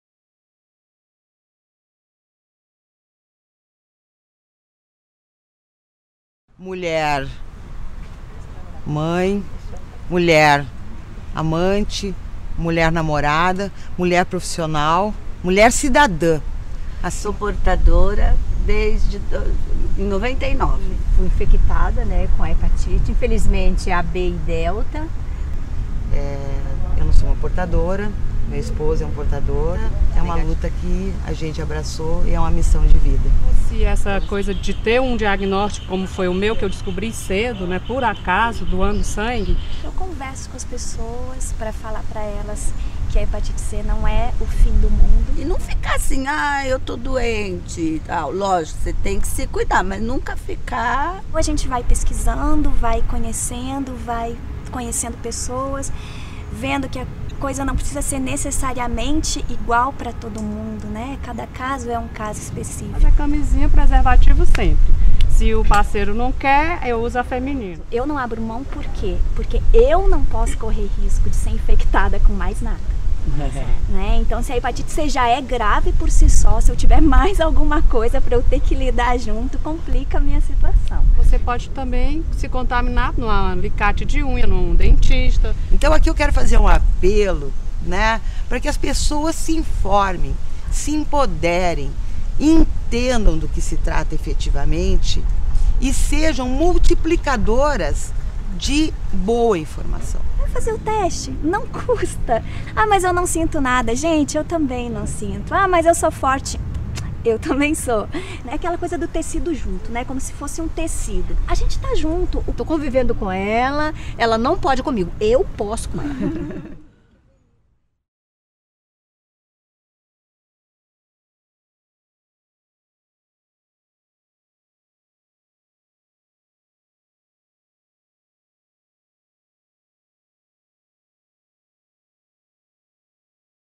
Break the silence of hepatitis - Women's International Day 2013
Video produced by the STD, AIDS and Viral Hepatitis Department to celebrate Women's International Day 2013.